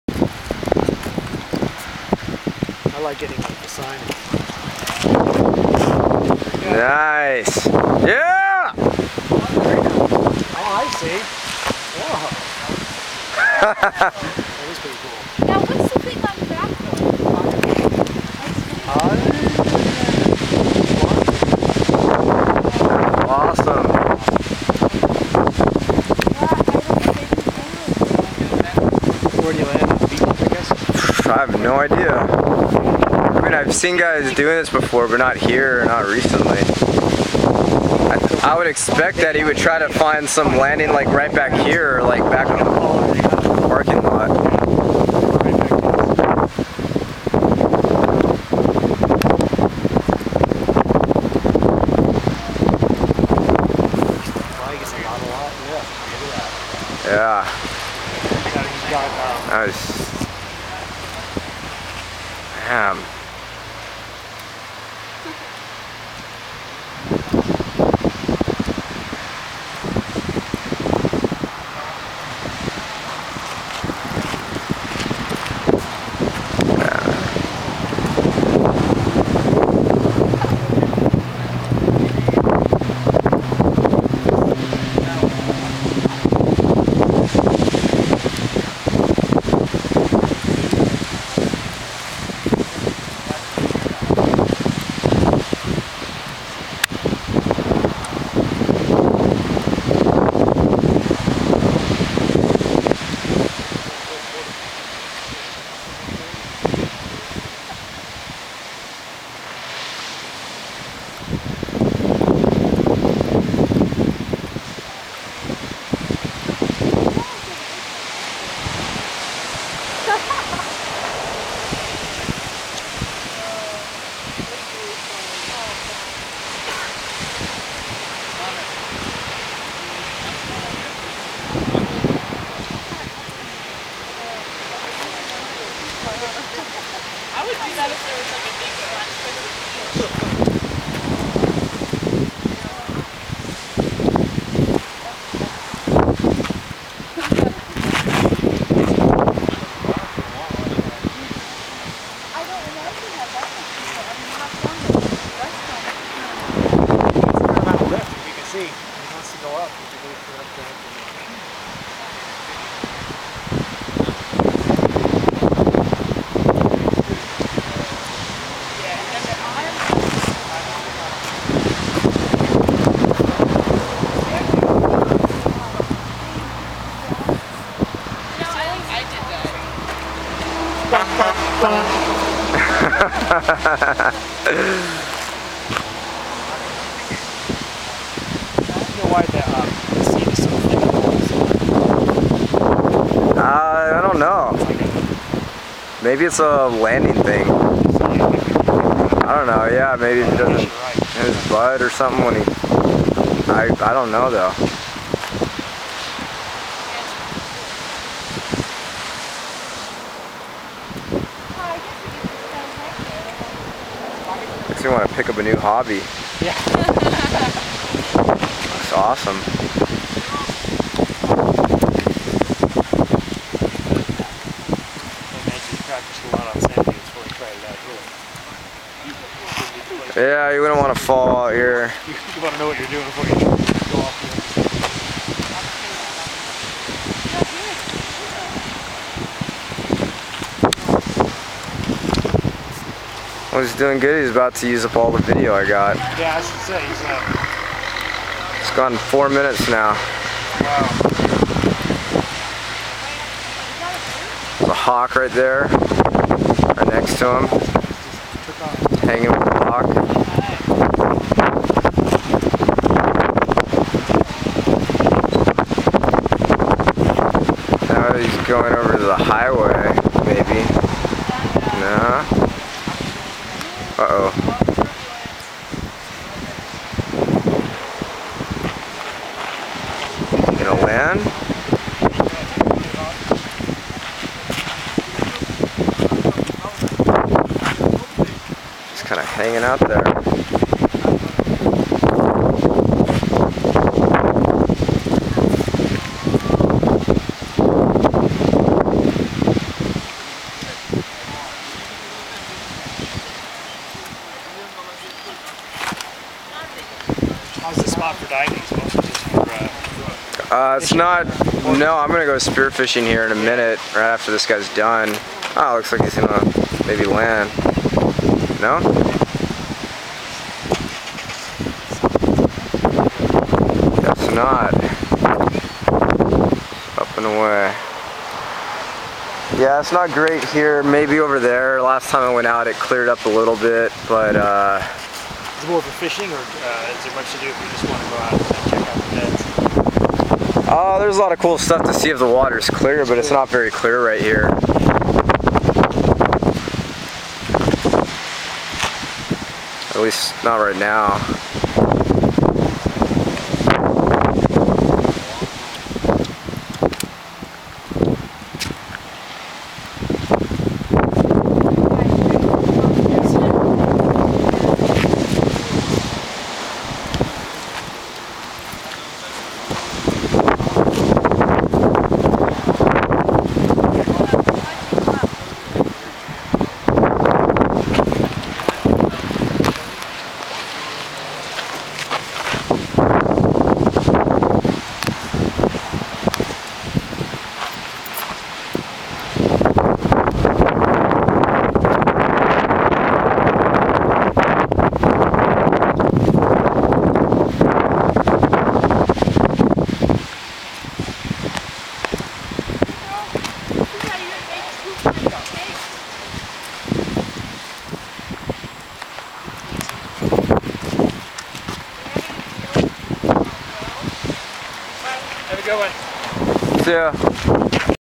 William Deaguiar Paragliding at Palos Verdes
William Deaguiar paragliding in Palos Verdes. I just happened to catch him taking off just before I went spearfishing. I spoke with him at the bottom of the hill and he told me he tried to land up top, but there was too much multi directional wind to land there. He ended up landing safely at the bottom of the cliff and had to hike back up Cardiac Hill. -Steve White